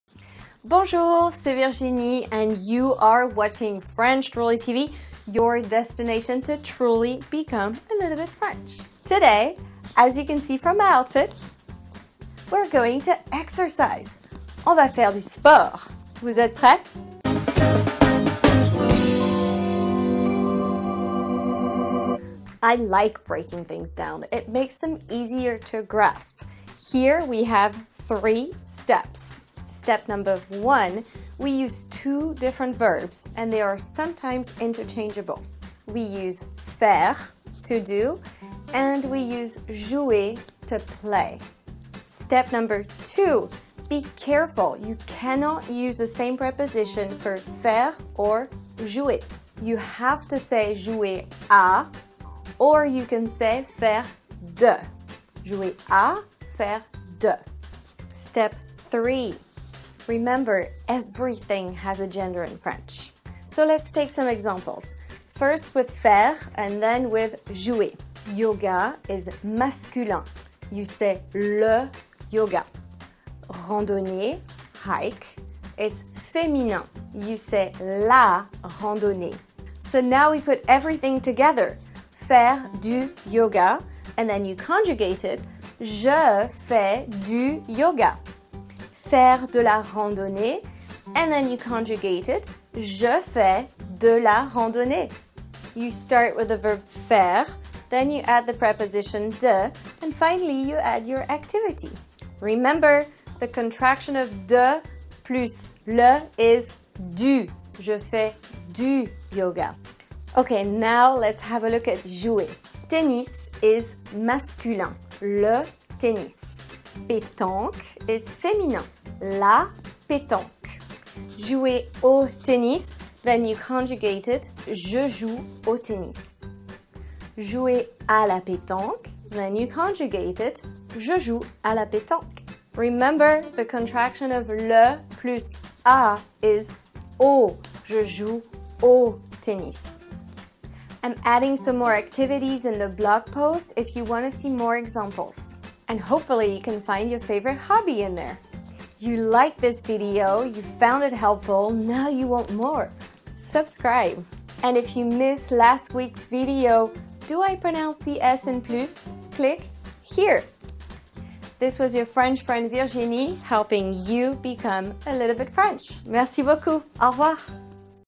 Le sport français
Talking about activities & sport in French can be a little confusing. Different verbs, different prepositions, different genders. How can I do it right? Here's how! I break it down in 3 steps for you.